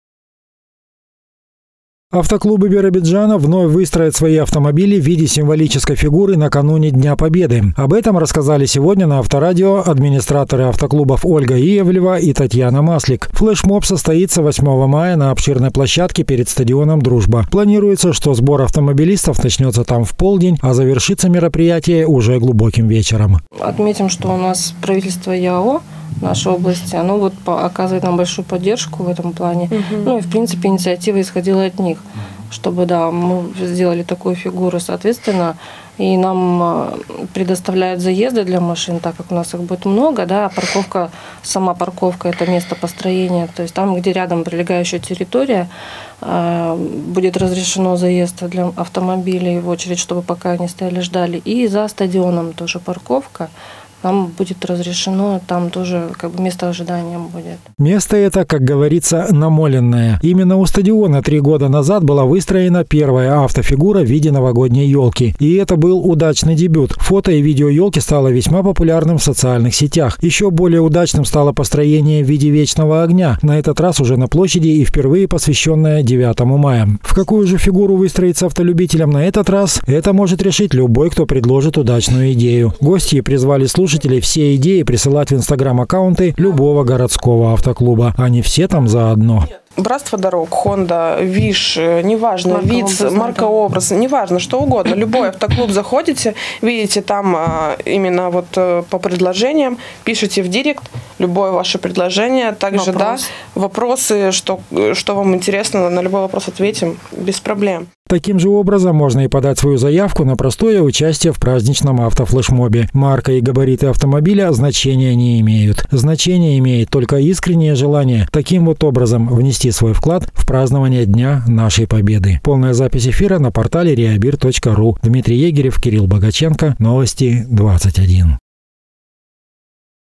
Автоклубы Биробиджана проведут флешмоб ко Дню Победы
Автоклубы Биробиджана вновь выстроят свои автомобили в виде символической фигуры накануне Дня Победы. Об этом рассказали сегодня на Авторадио администраторы автоклубов  Ольга Иовлева и Татьяна Маслик.  Флешмоб состоится 8 мая на обширной площадке перед стадионом «Дружба». Планируется, что сбор автомобилистов начнётся там в полдень, а завершится мероприятие глубоким вечером.